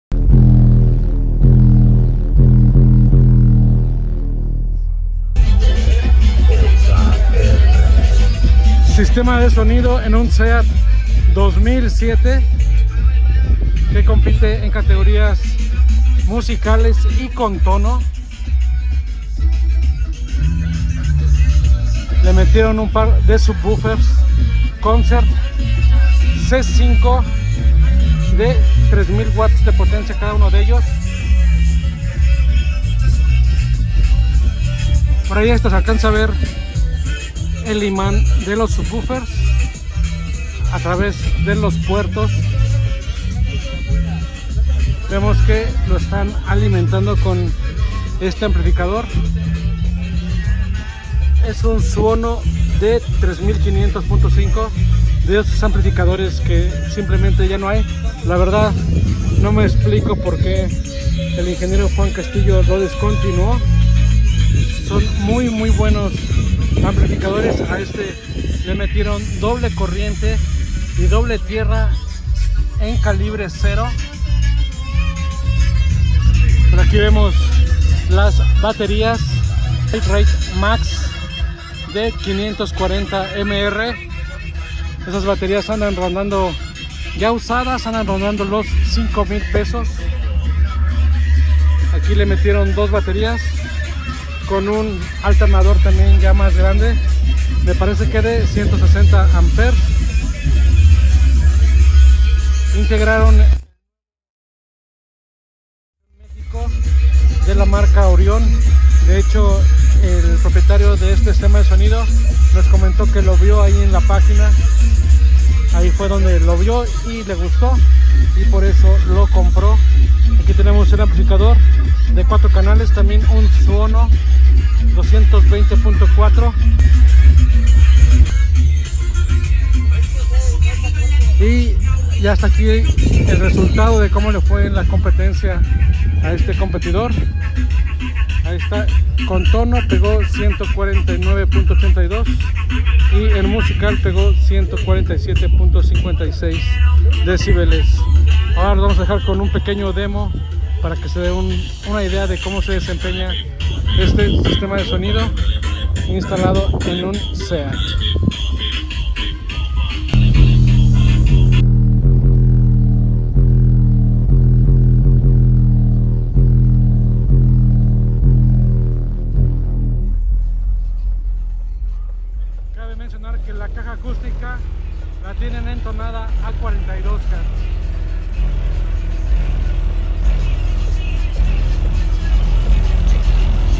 Subs Concert de 3K con Amplificador SUONO
En esta ocasión le bajó un poco el número al competidor, pero sin duda cuenta con buenos componentes para SPL.
Lo grabamos en el 3er Performance Fest del Bass performance Team